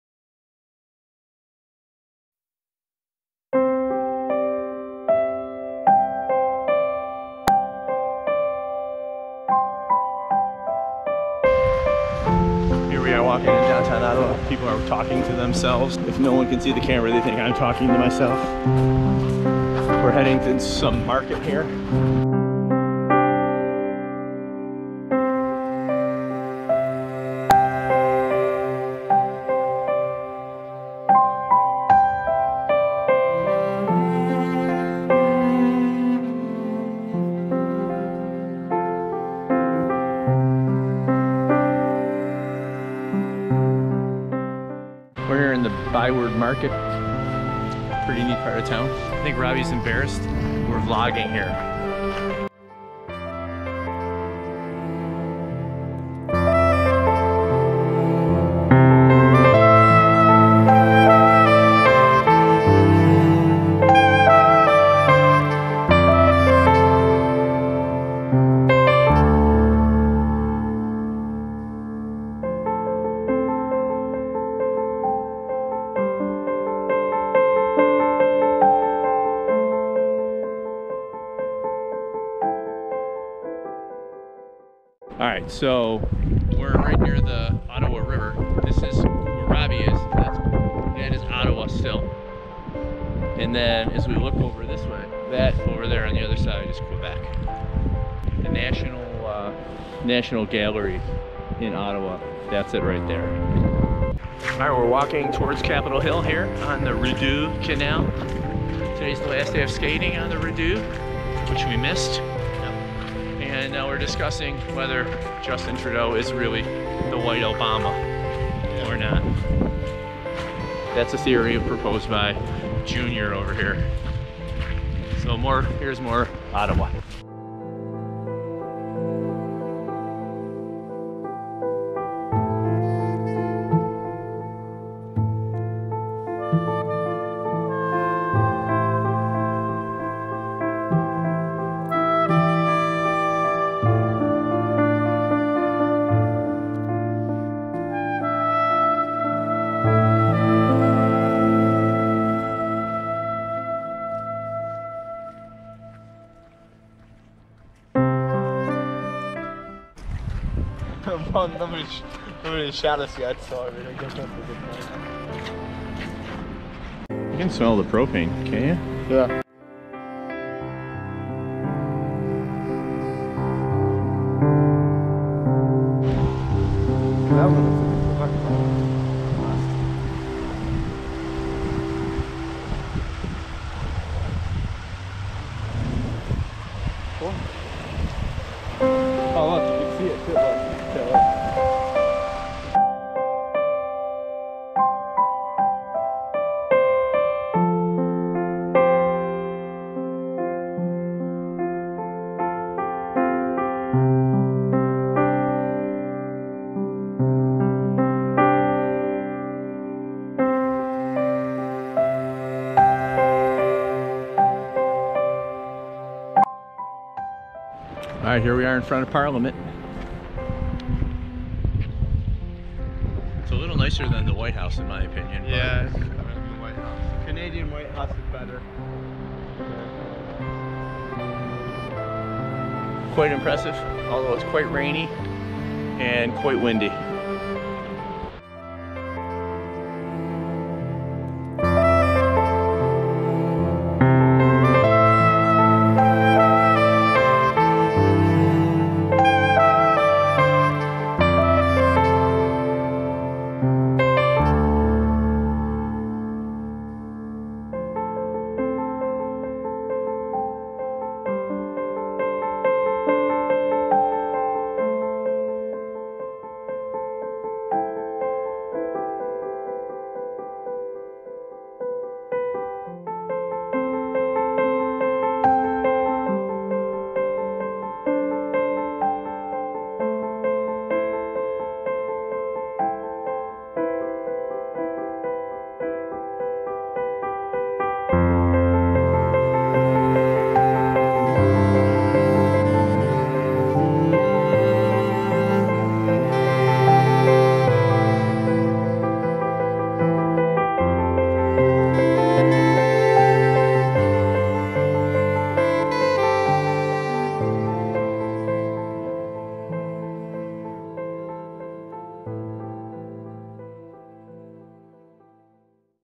Ottawa Day One
3/10/19 Ottawa, Canada
In town for Rob's skate with the Ottawa Junior Senators. More to come for day 2 and day 3.